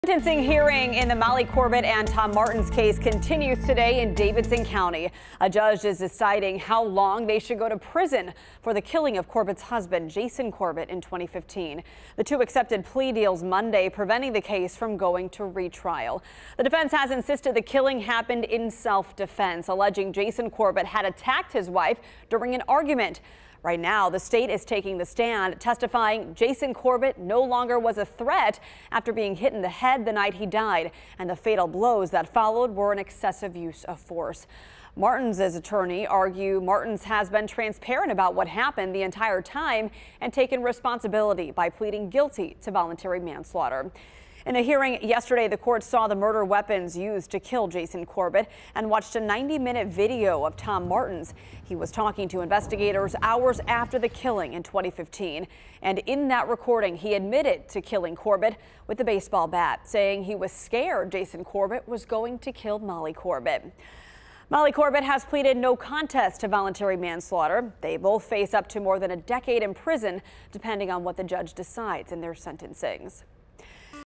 Day 3: Corbett, Martens sentencing hearing continues
Molly Corbett and Tom Martens both face more than a decade in prison depending on what the judge decides in their sentencings.